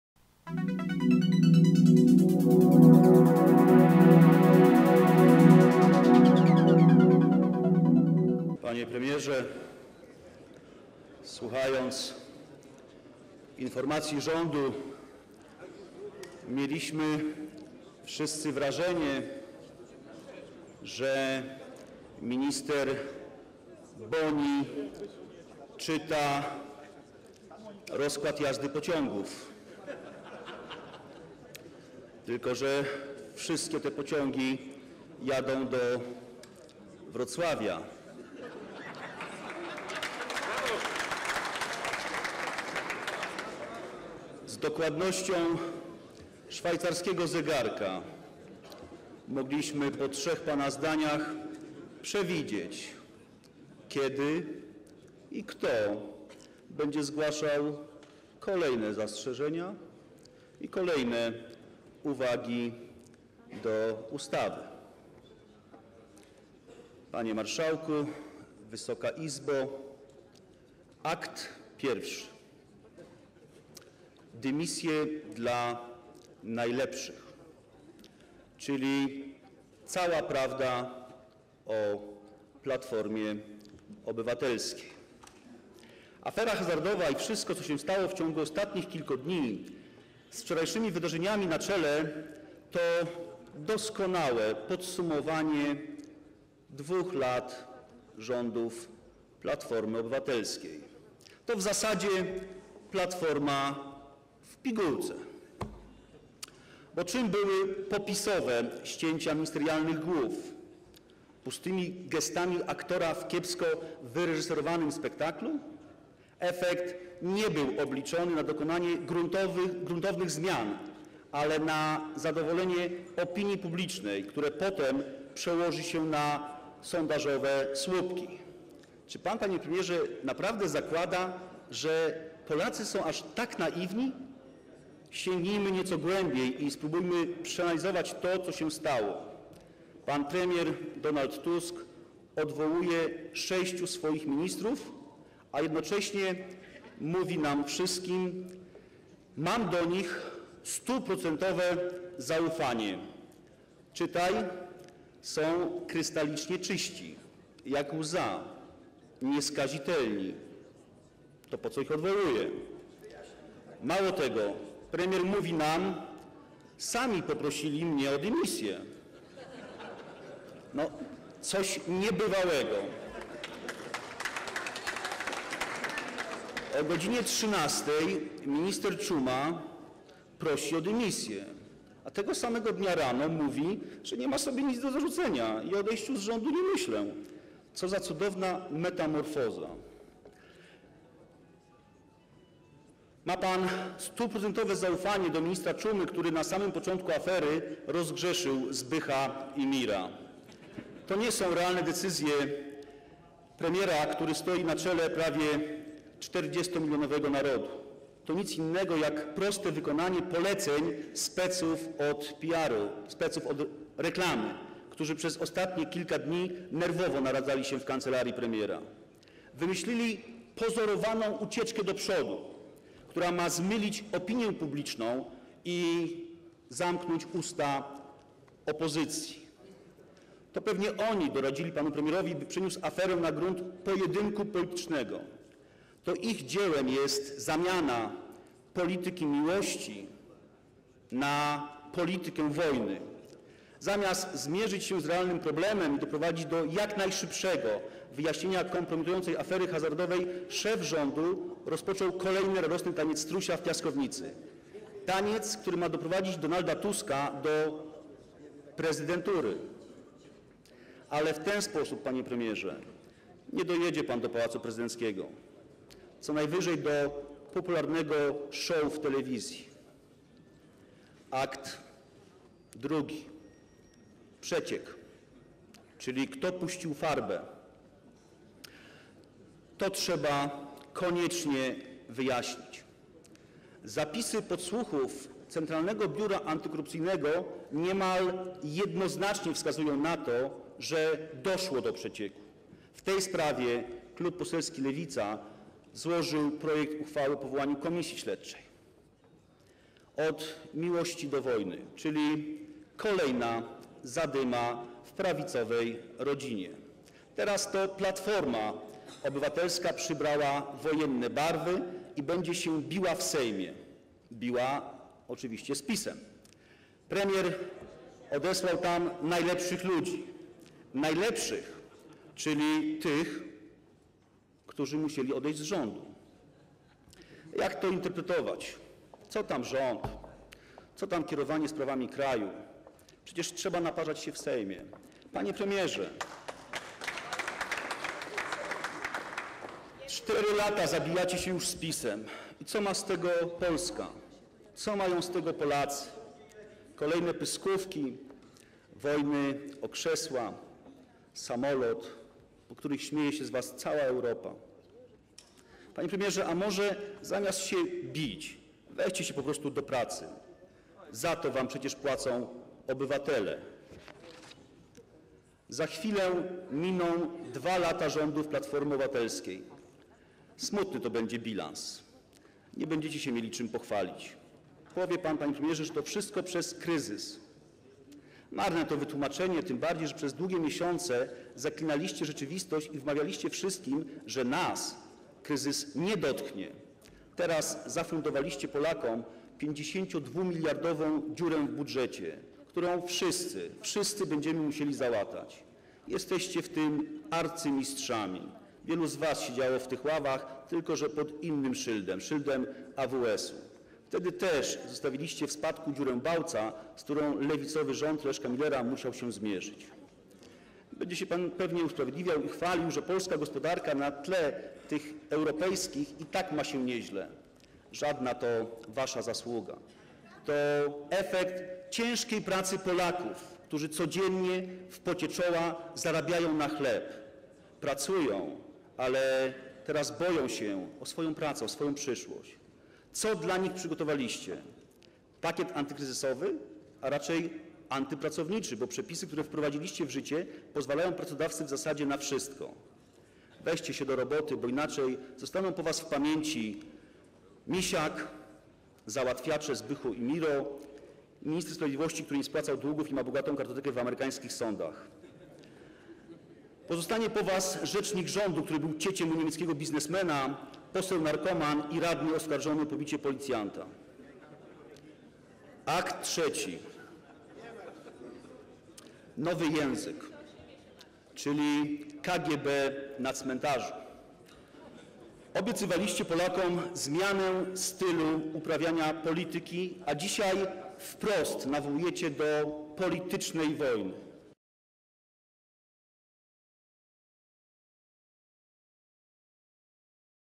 TVSLD : Dramat w V aktach - Marek Wikiński cz 1
Wystąpienie Marka Wikińskiego, wiceprzewodniczącego Klubu Poselskiego Lewica podczas debaty w Sejmie nad rządową informacją o przebiegu prac nad projektem zmian w tzw. ustawie hazardowej